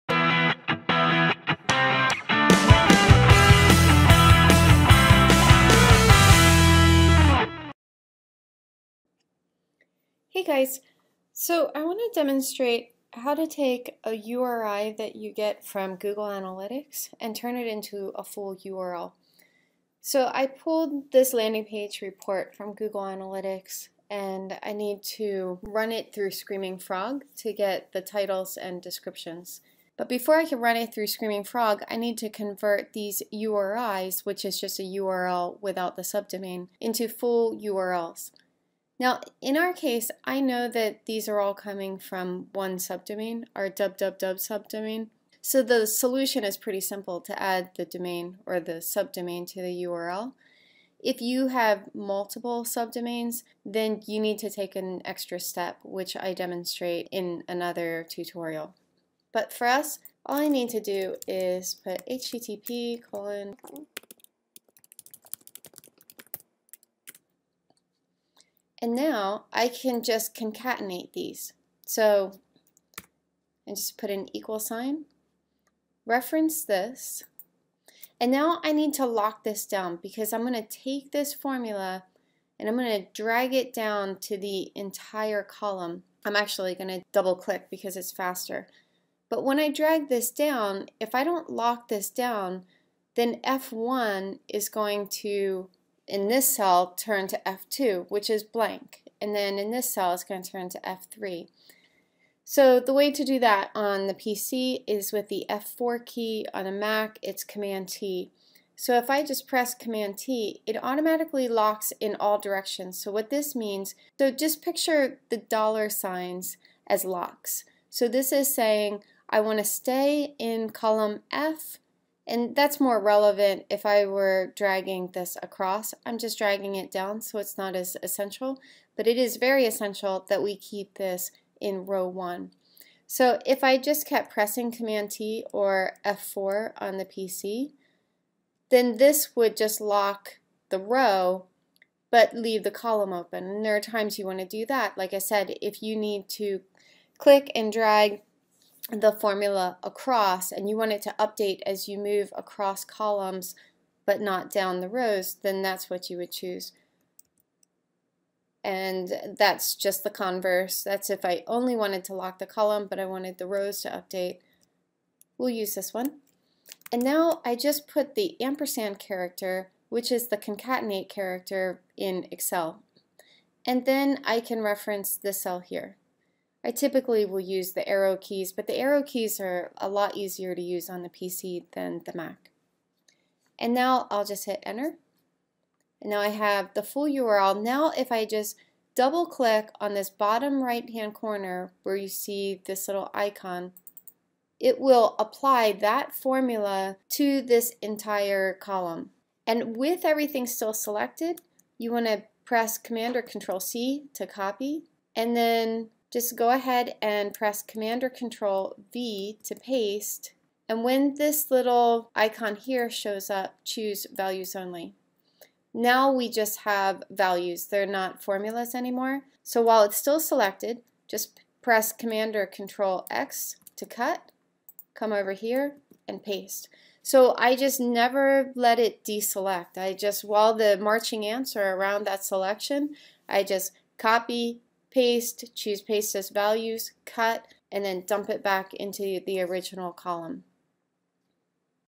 Create Full URLs from Google Analytics in Excel or Google Sheets
Google Analytics uses URIs instead of URLs by default. But that makes it difficult to marry your data with data from other marketing tools. This video will show you how to create full URLs from Google Analytics content reports, whether your site uses one subdomain or multiple subdomains. This process is called concatenation and could also be accomplished with the CONCATENATE function, but I find it clunky. Although I demonstrate how to do this in Excel, the steps are exactly the same in Google Sheets.